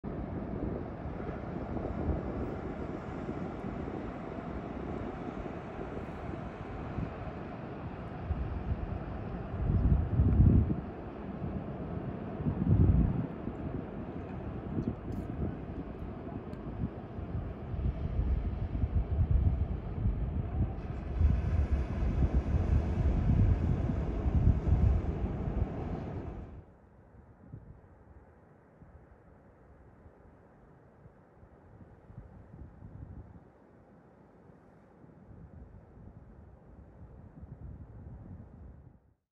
Okay, Elephant Snails are adorable (especially the teeny little ones 🥰)
Elephant Snails, Scutus antipodes, are super common in rock pools around the Illawarra and I usually find them hiding under rocks. The shell isn't a full shell that you might expect but rather a reduced shell and is often tucked away under the folds of the black mantle appearing only as a little strip of white if at all.